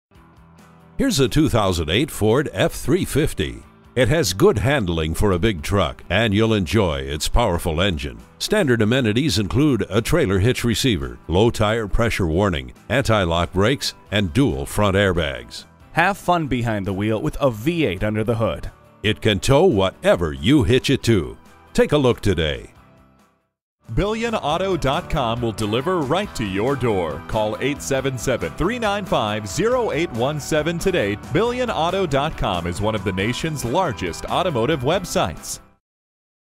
Used 2008 Ford SUPER DUTY F-350 DRW Rapid City Car- For- Sale, SD
Year: 2008
Make: Ford
Model: SUPER DUTY F-350 DRW
Trim: Lariat
Engine: V8
Transmission: Automatic
Color: Black
Interior: Medium Stone
Mileage: 99560
Stock #: D14420B
VIN: 1FTWW33R98ED40928

Address:
19 Midwest Locations